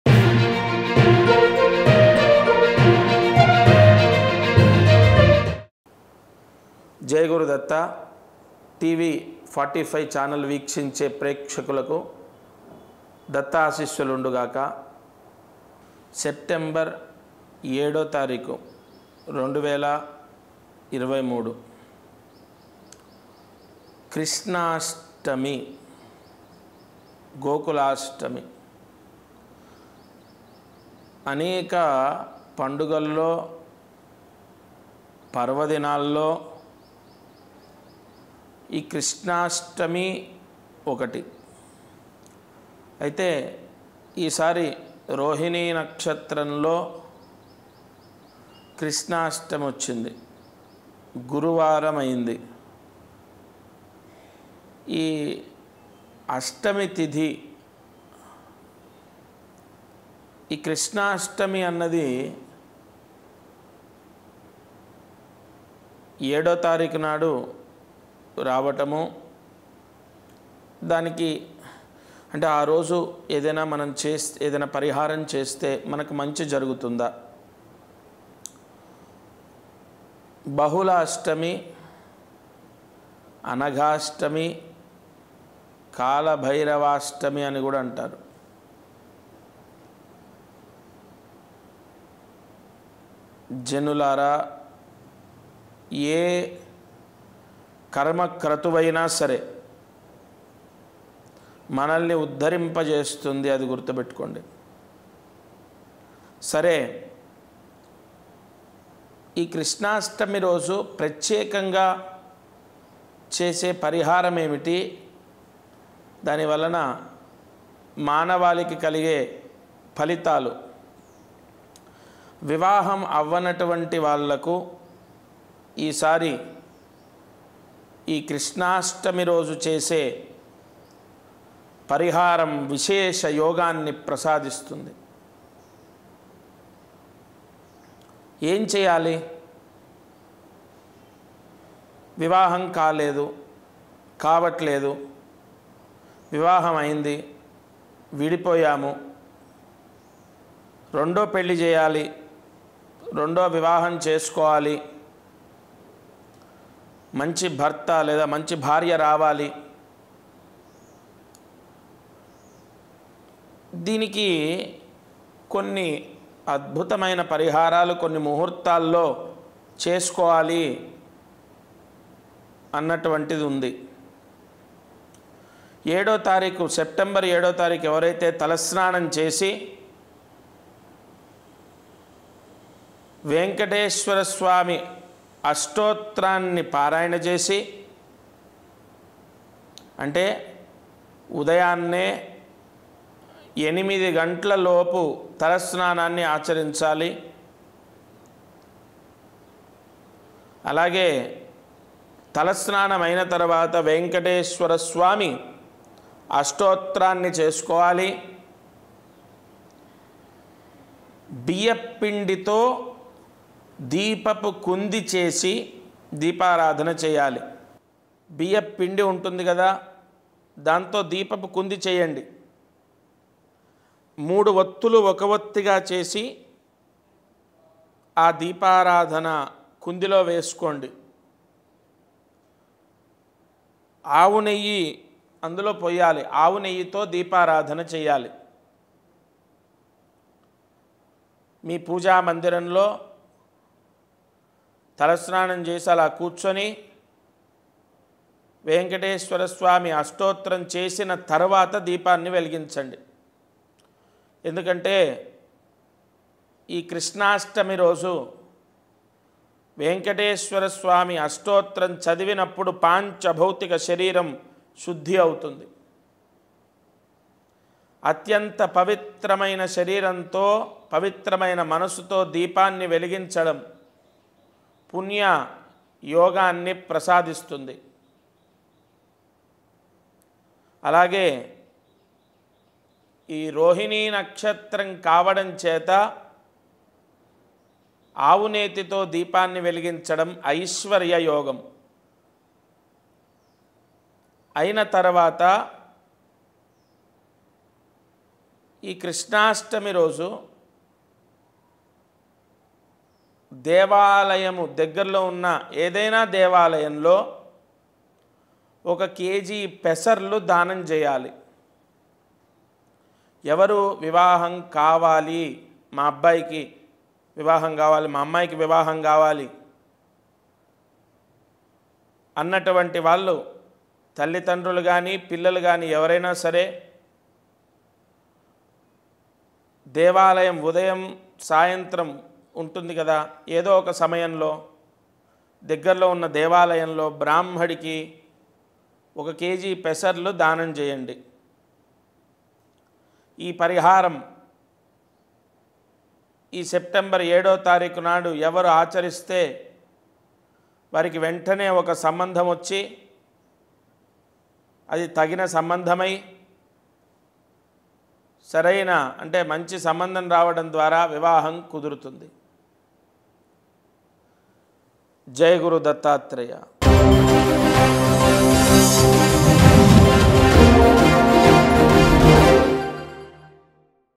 Astrologer Lakshmi Kanth Sharma About Krishnashtami On 7- September -2023
7- September -2023 కృష్ణాష్టమి రోజు ఇవి చేయండి శుభవార్త వింటారు